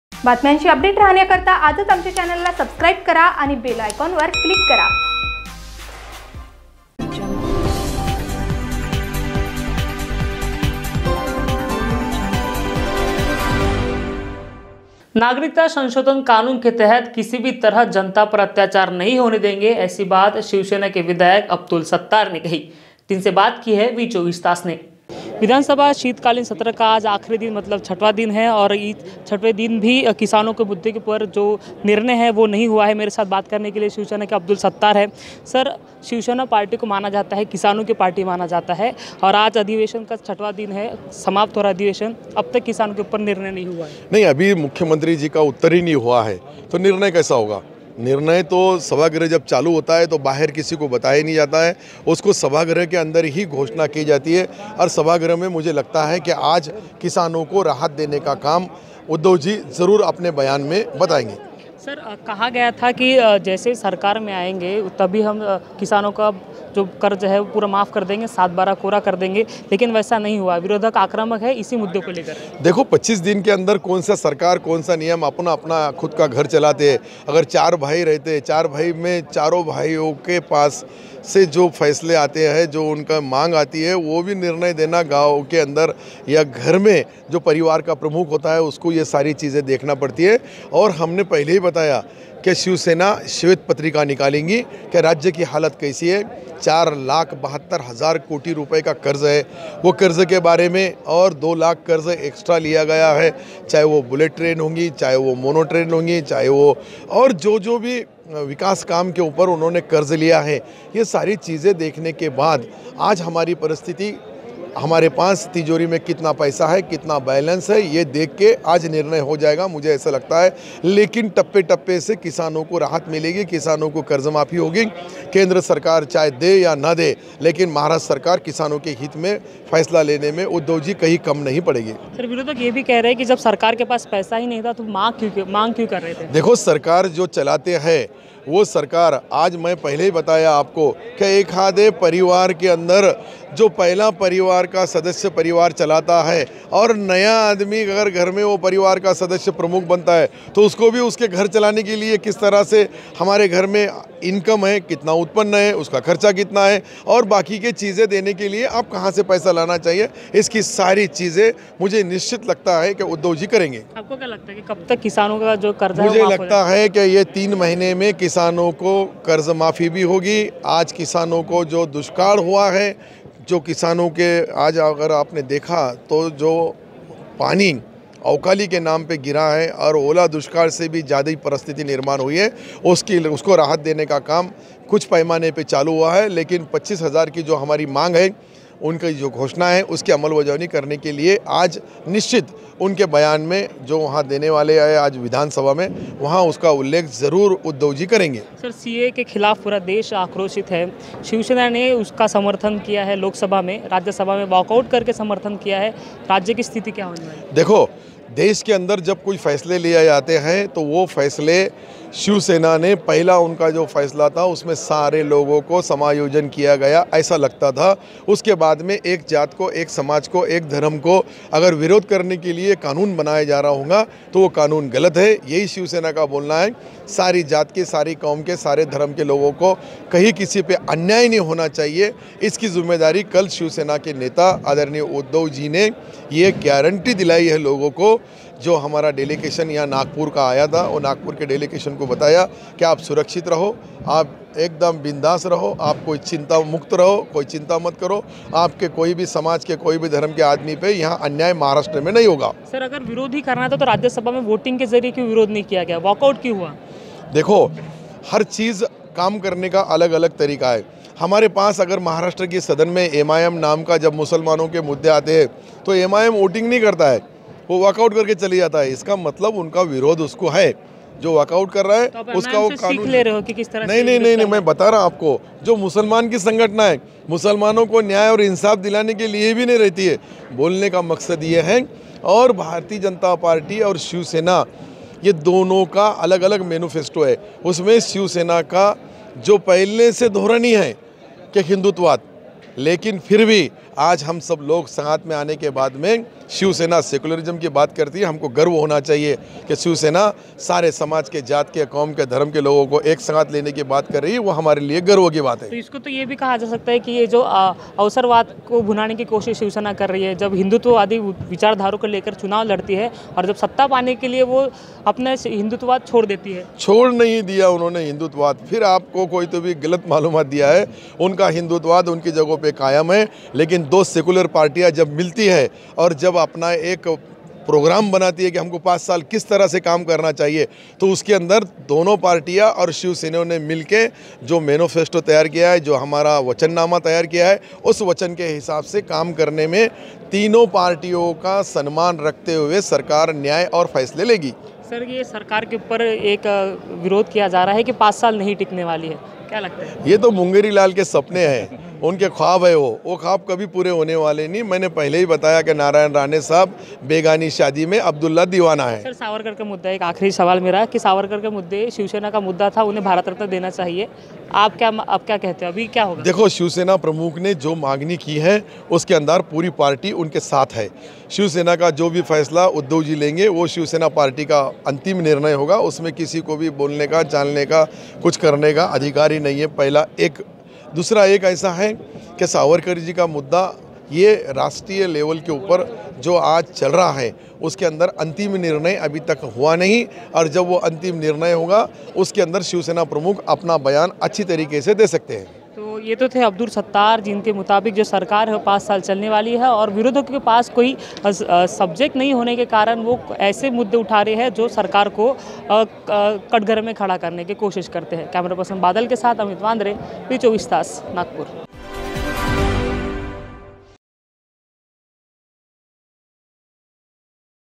शिवसेना विधायक अब्दूल सत्तार से व्ही २४ तास से बातचीत I V24 Taas I 21 Dec 19
सी ए ए के तहत किसी से भी दुजा व्यवहार नही होगा   I V24 Taas I 21 Dec 19
शिवसेना विधायक अब्दूल सत्तार से व्ही २४ तास से बातचीत